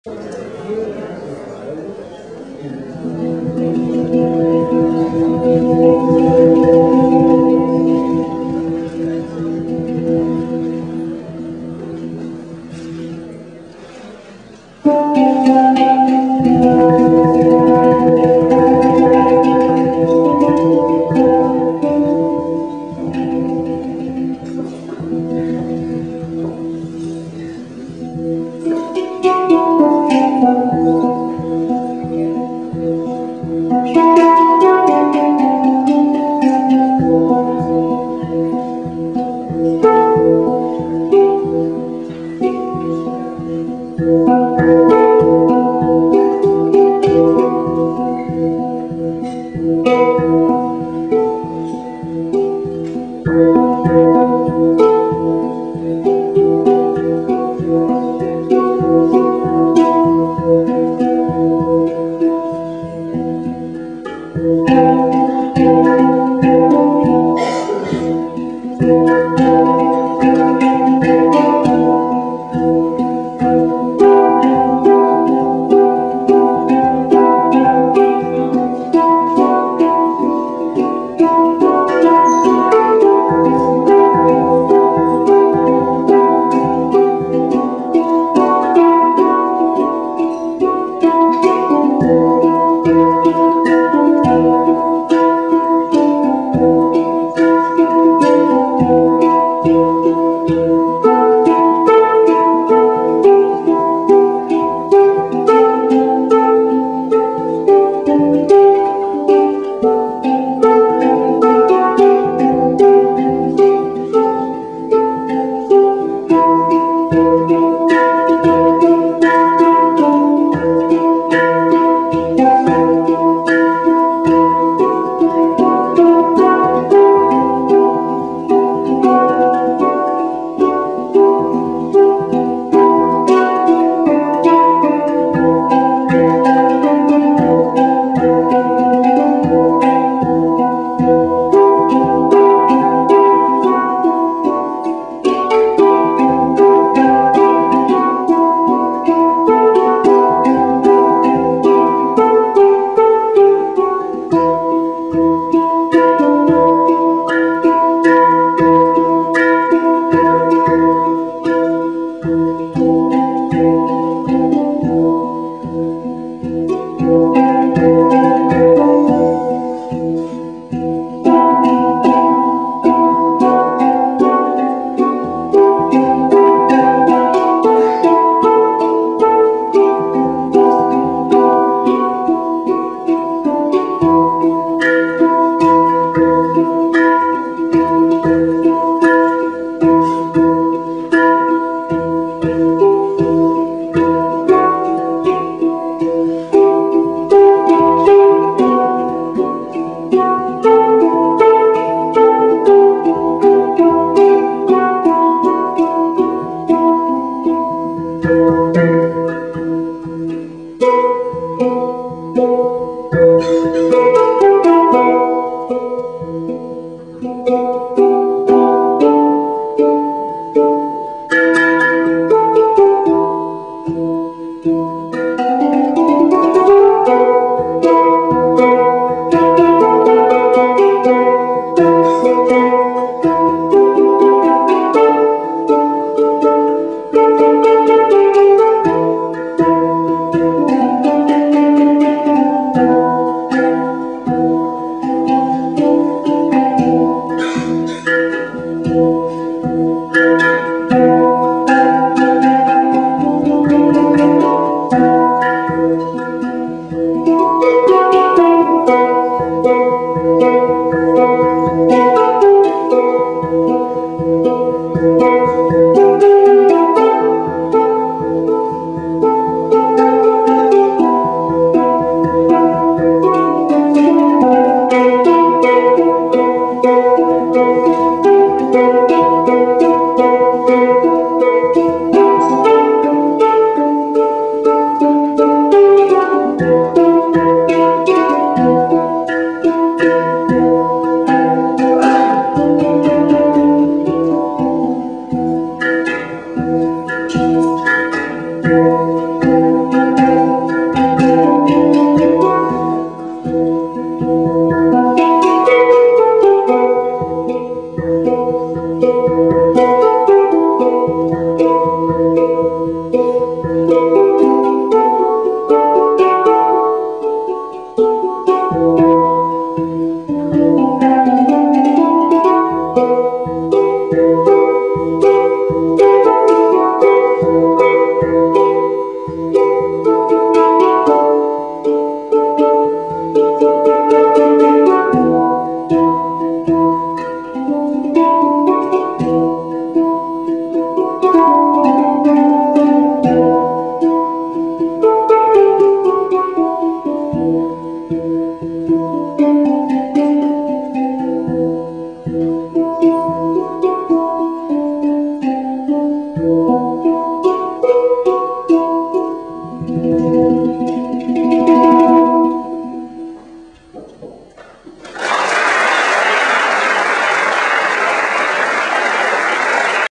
Hang for Dan Millman
This Hang composition by Joshua Samson was performed before the lecture of inspirational speaker Dan Millman